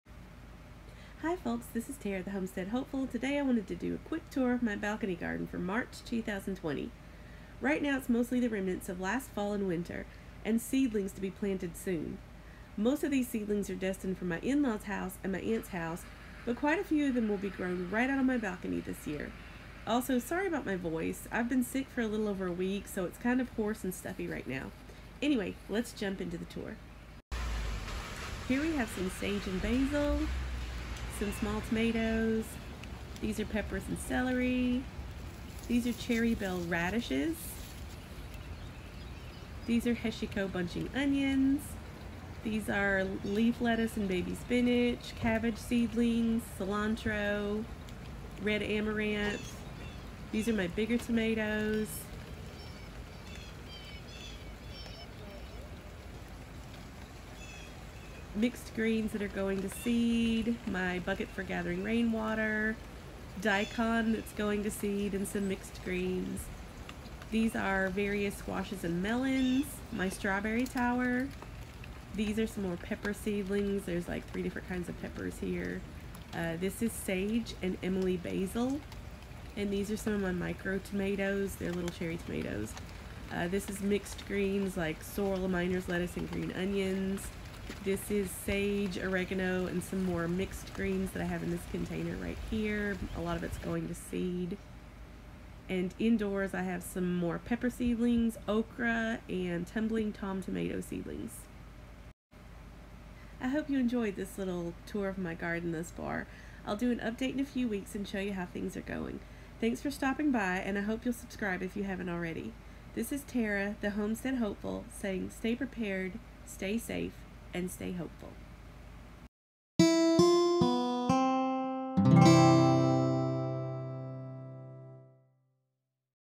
Small Space Balcony Garden Tour Spring 2020!
This is a tour of my small space balcony garden at the end of March 2020!